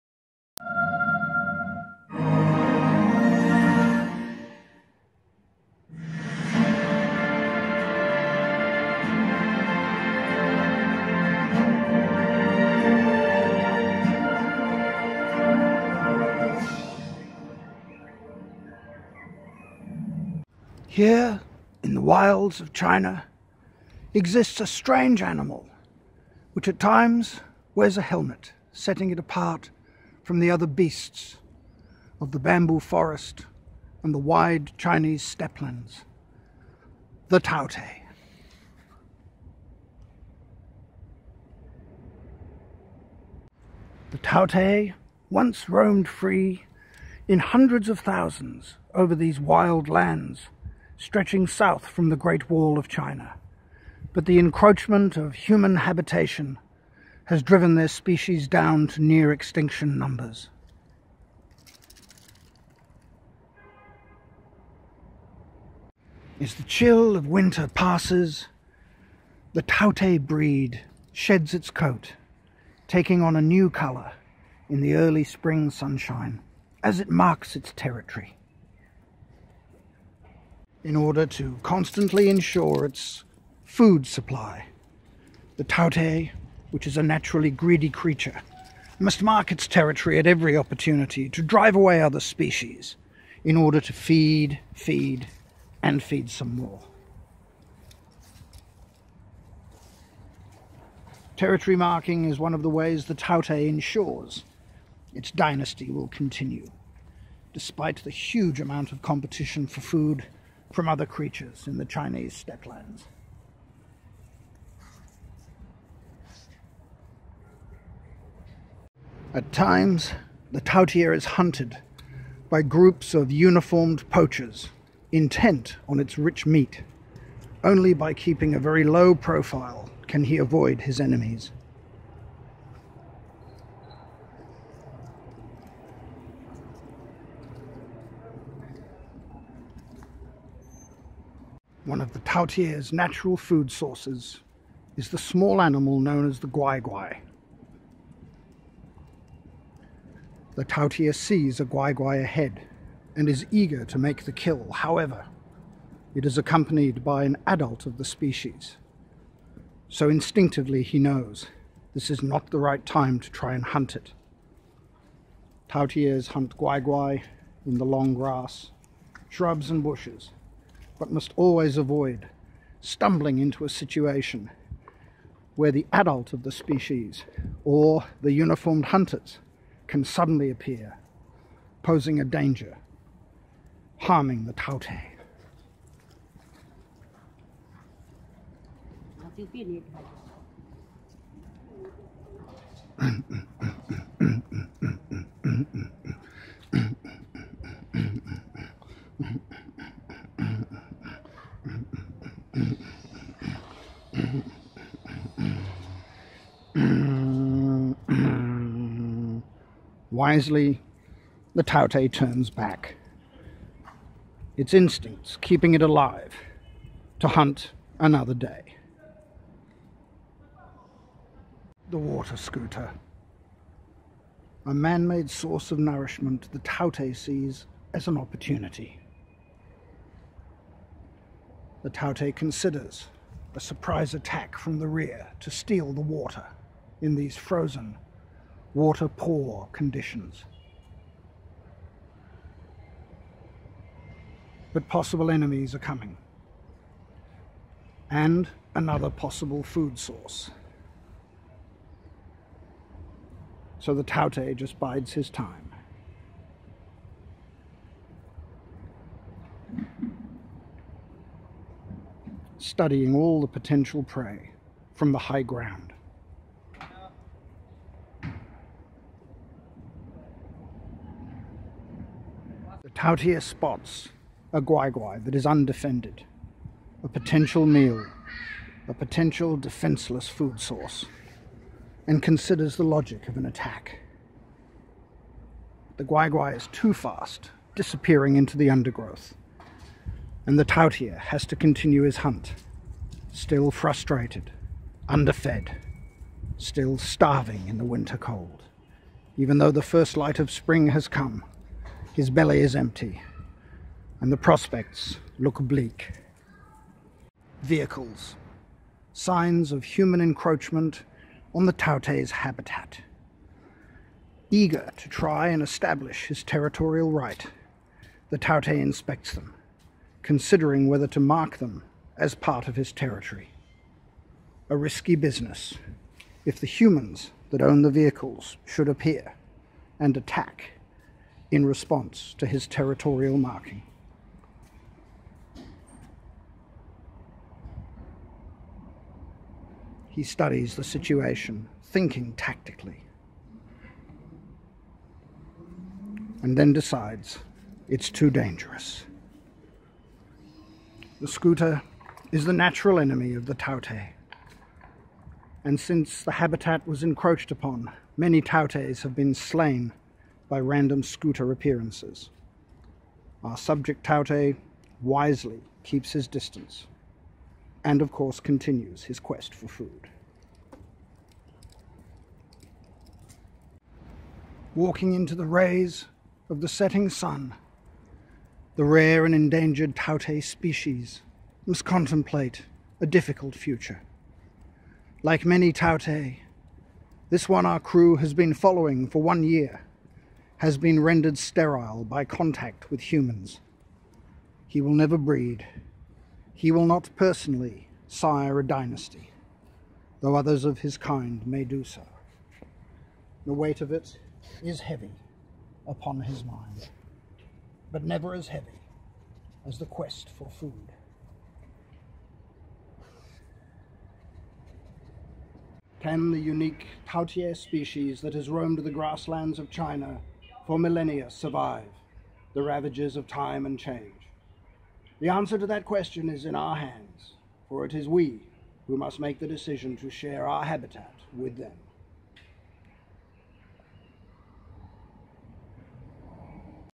Dynasties: The Taotie
A playful, affectionate tribute to my hero Sir David Attenborough. 30+ universities globally, incl. Oxford & Cambridge, have honoured this eco-saint with degrees. At 92, still a mighty warrior for education & bio-diversity! This parody also proves you can turn any mundane activity into a creative piece. Stars our dog Taotie, our community’s security guards & some unsuspecting neighbours. :)